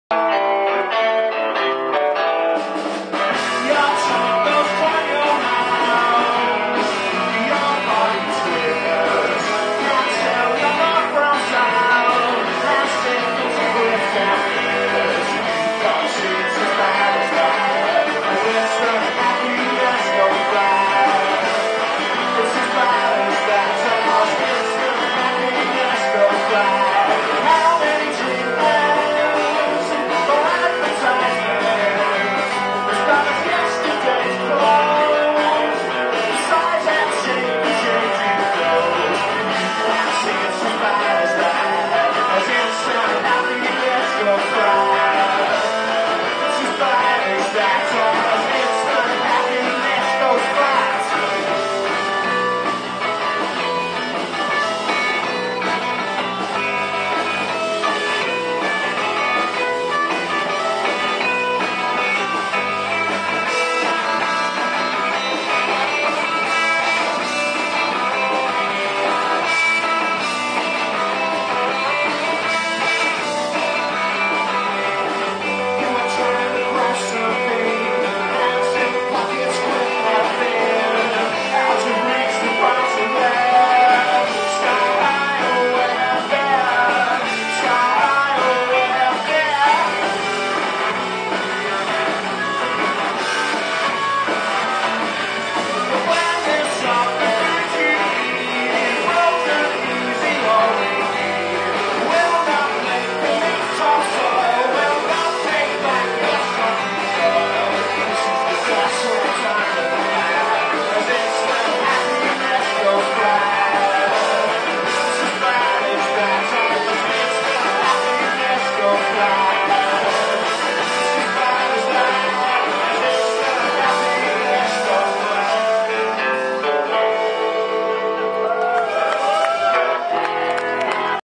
The Wolfhounds - Gullivers Manchester 27/10/12
The Wolfhounds live at Gullivers Manchester as part of the Un-Peeled event.